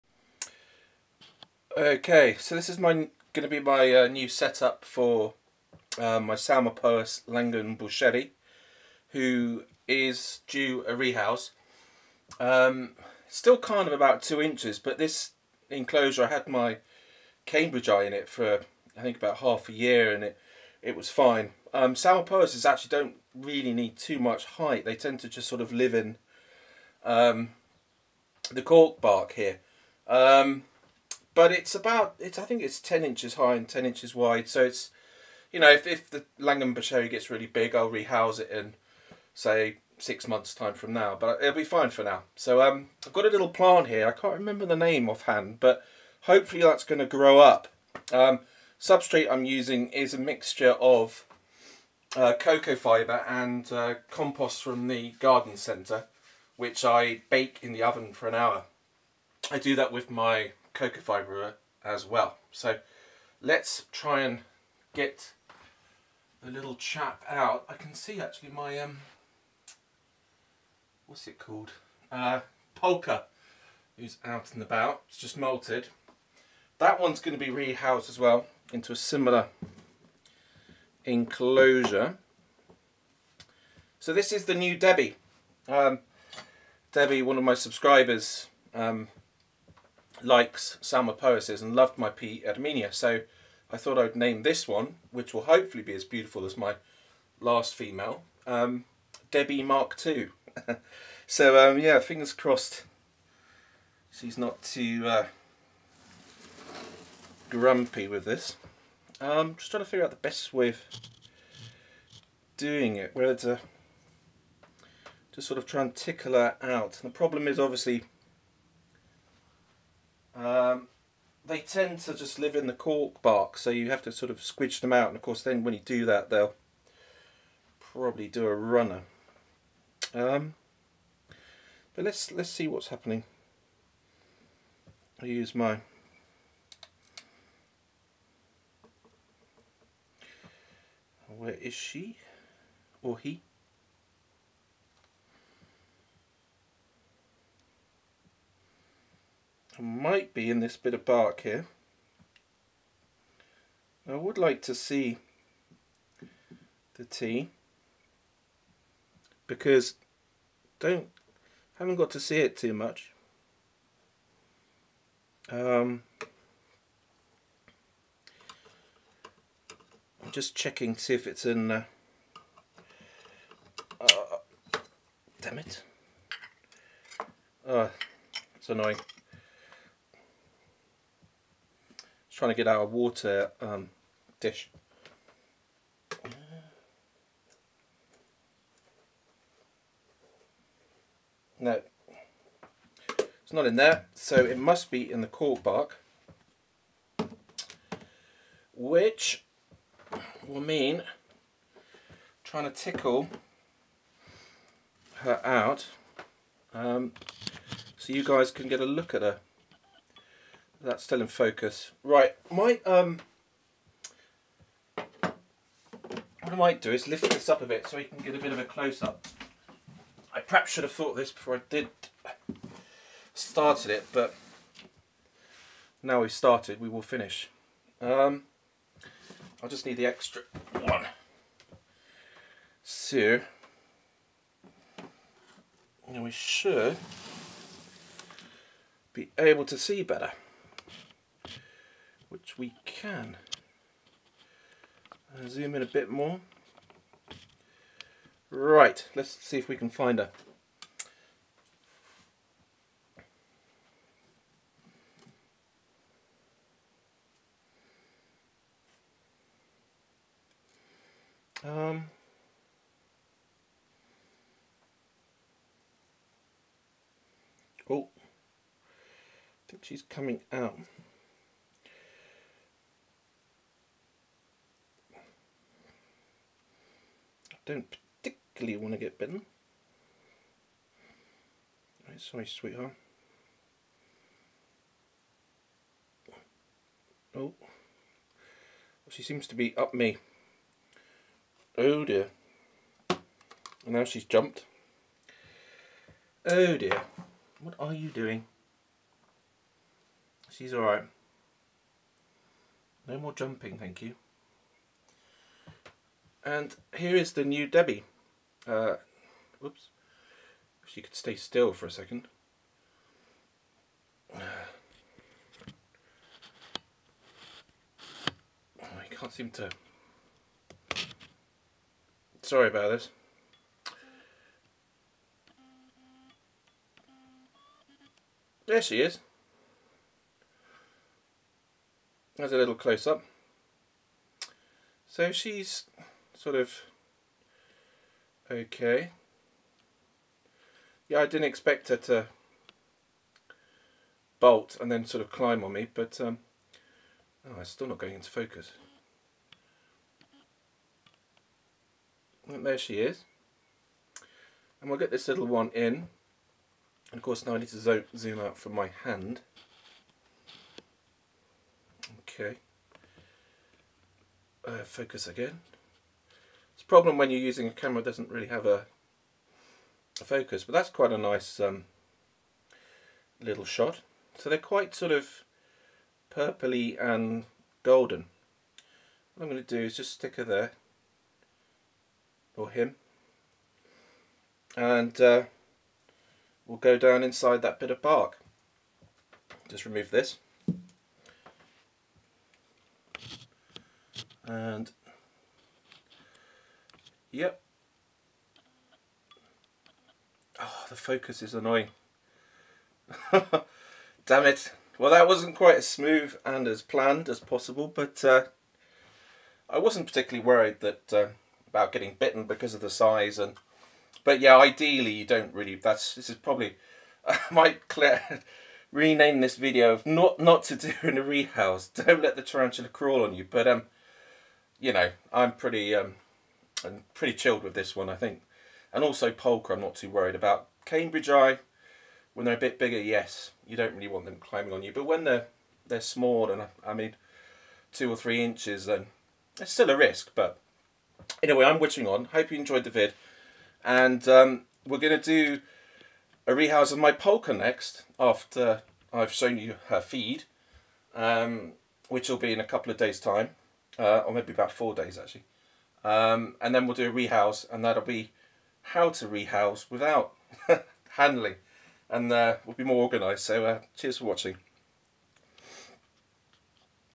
Debbie Mk.2! Rehousing (not textbook) my P Langenbucheri
So messed it it up a tad. But T is ok!

Hopefully P Pulcher goes better prob weekend depending on whether he/she eats. Oh and dont know the sex of my Langenbucheri yet not sure why I kept on saying she.

Lets hope havent jinxed it